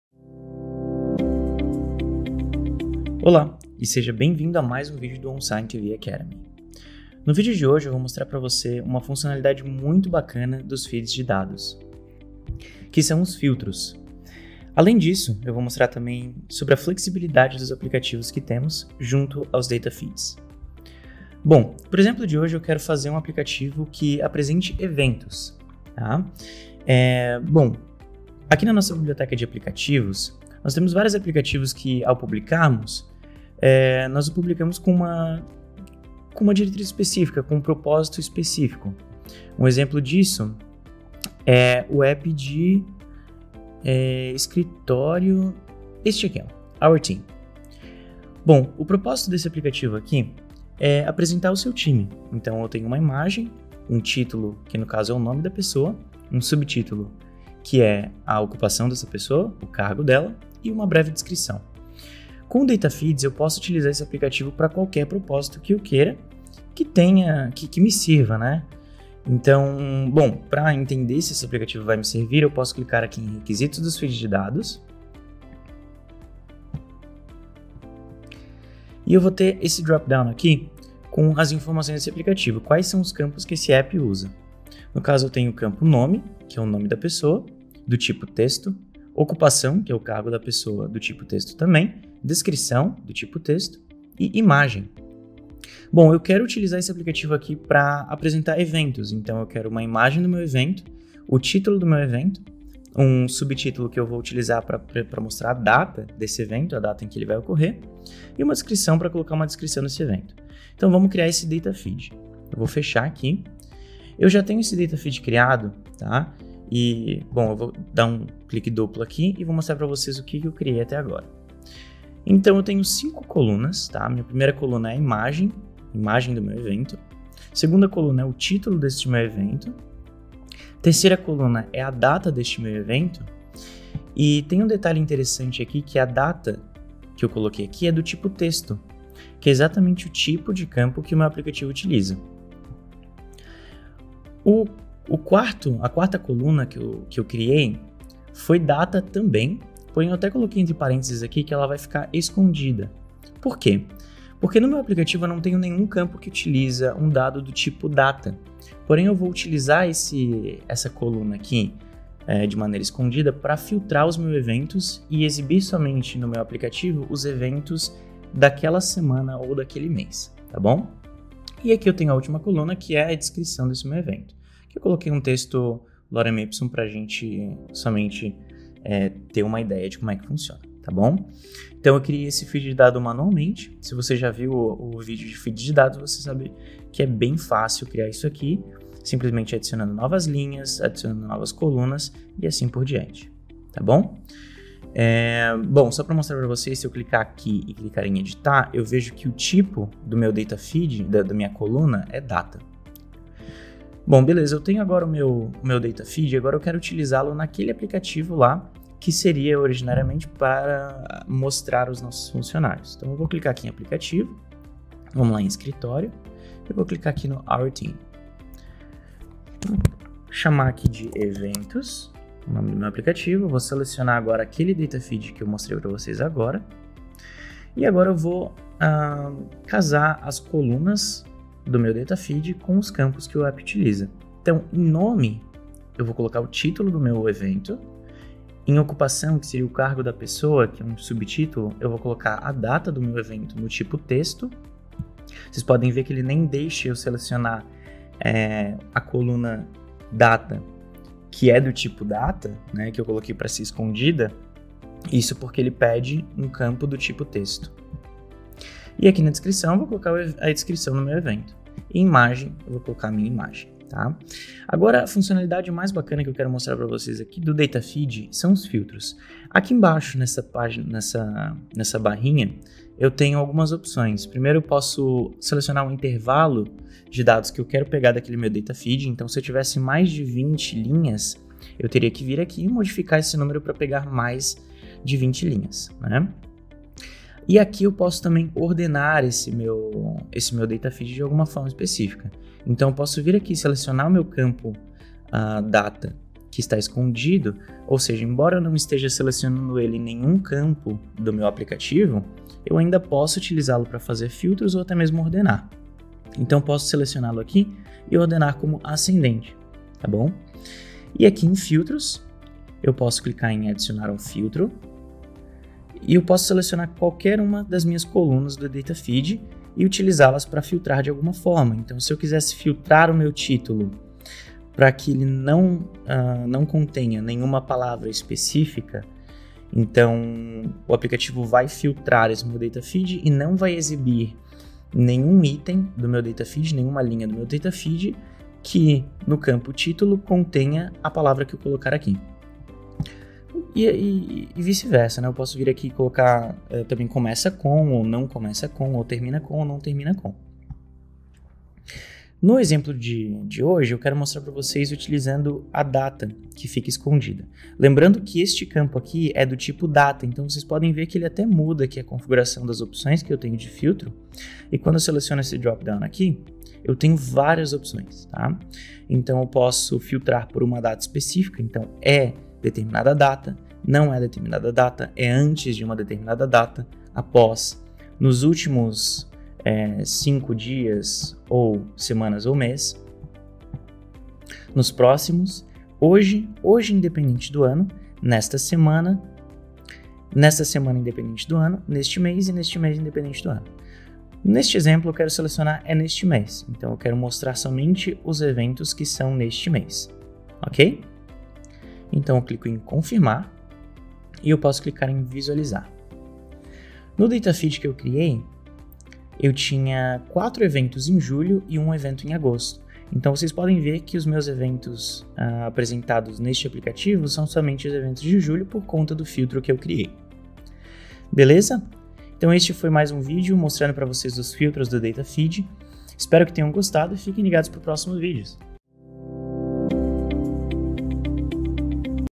16 - Como aplicar filtros nos Feeds de dados - OnSign TV Academy (Português)
Os filtros nos feeds de dados especificam quais dados devem ser incluído ou excluídos de seus aplicativos.

Teste o software de sinalização digital OnSign TV. Visite nosso site e crie seu teste gratís de 14 dias